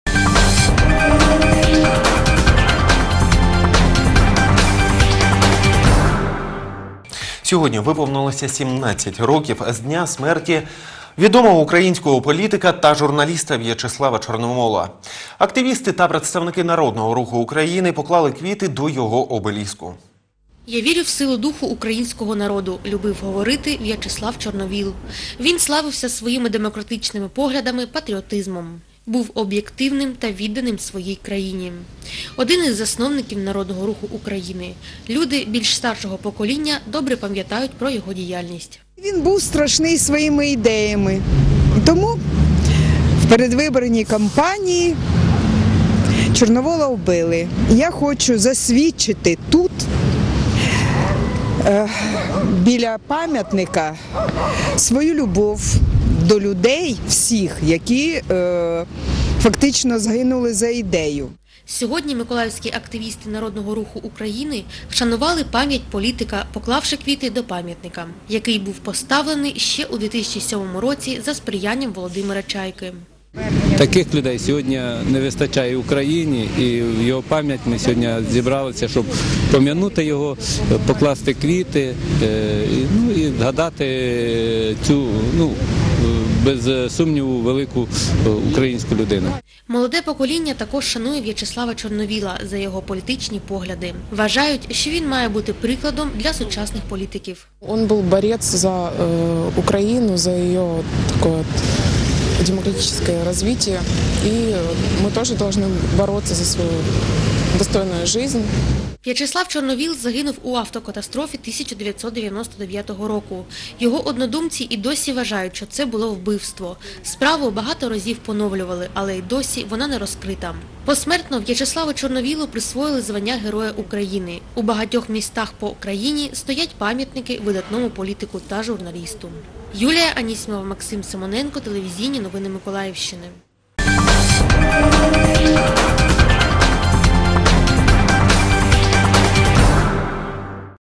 ТНМ сюжет — пам’яті журналіста В’ячеслава Чорновола
25 березня 2016 року виповнилось 17 років з дня смерті відомого українського політика, журналіста В’ячеслава Чорновола.
Автор — Юлія Анісімова.
Оператор — Максим Симоненко.